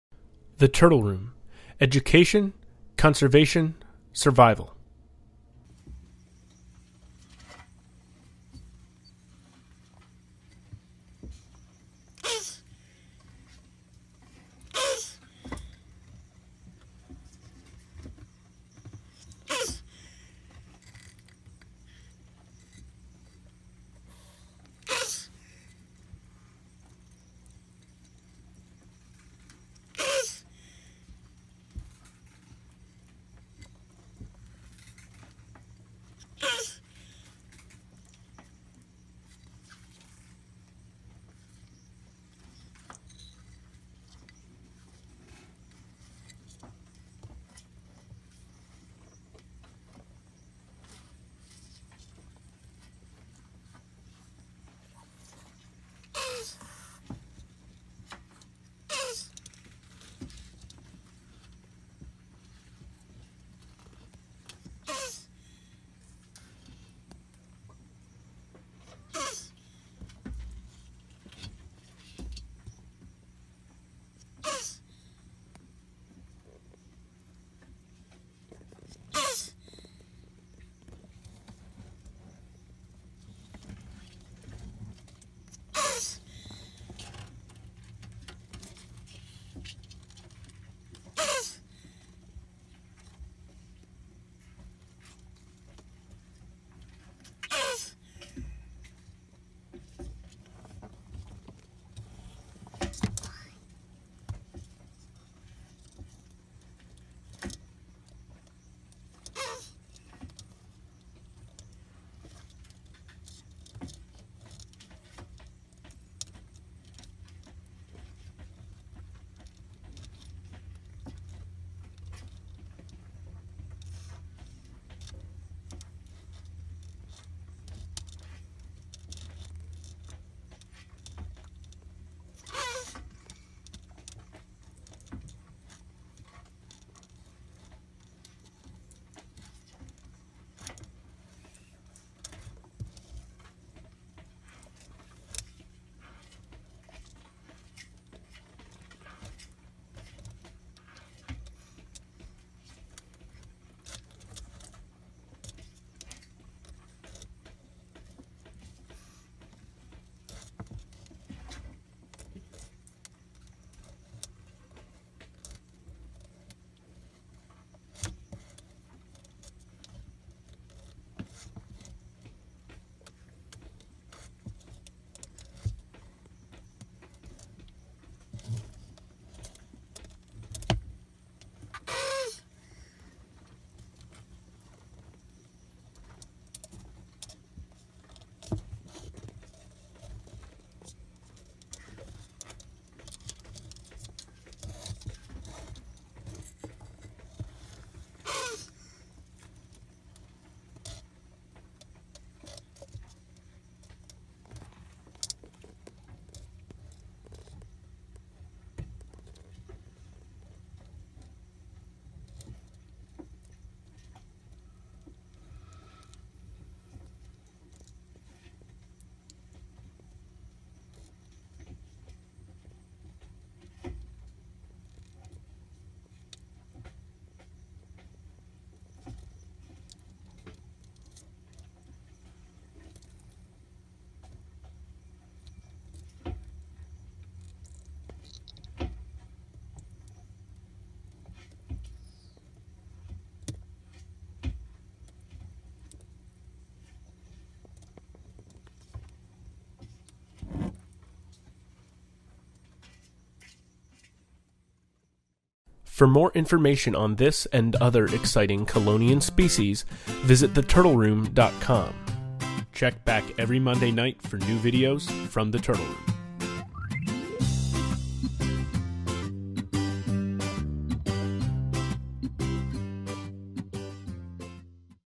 GRAPHIC! Box Turtle Breeding - Mature Audiences Only
WARNING! This video is graphic but is also important for scientific purposes. This type of footage is rare for any species, and is entirely non-existent for this rare species.

Chris Leone captured this impressive footage of his pair of Bourret's Box Turtles (Cuora bourreti) breeding at Garden State Tortoise.